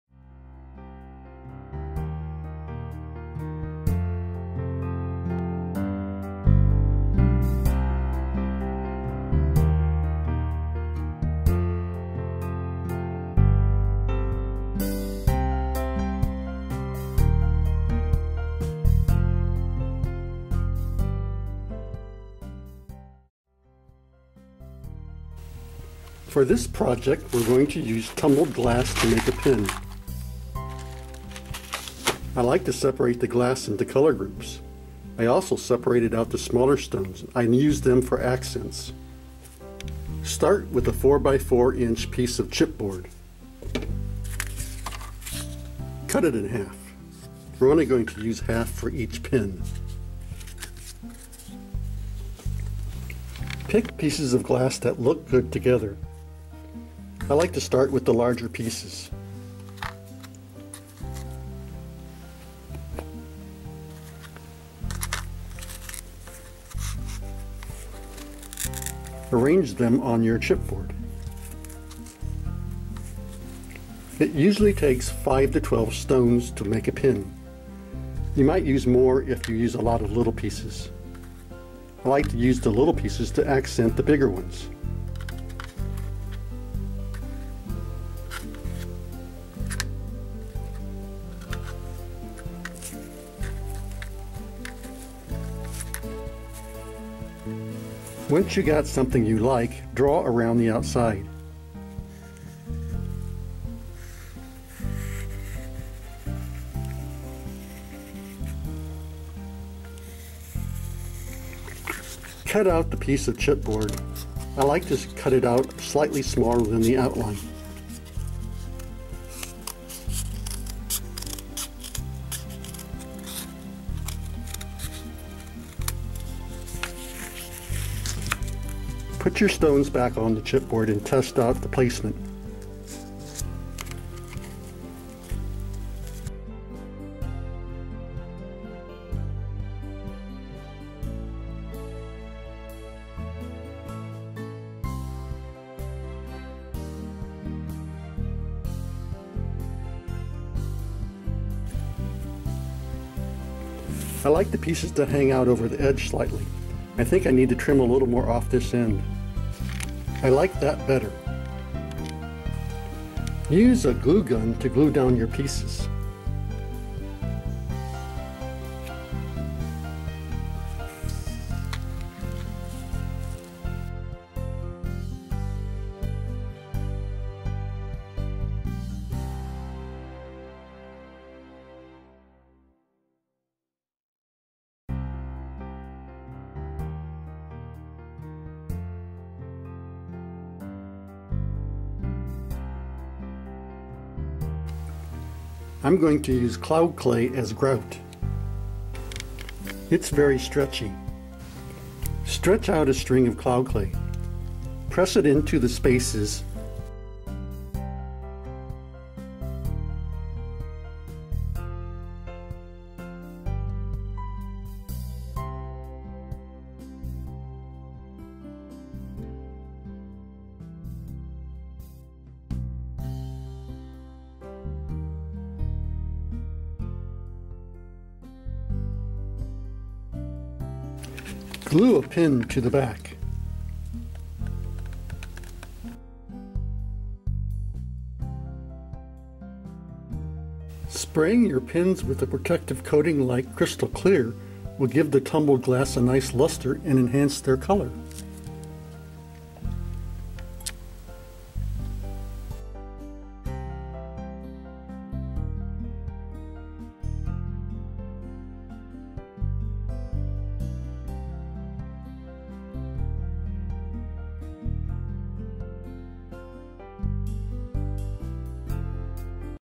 Tumbled Glass Pin - Project #178
United Art and Education Original Art Project: Make a cool pin with tumbled glass and cloud clay!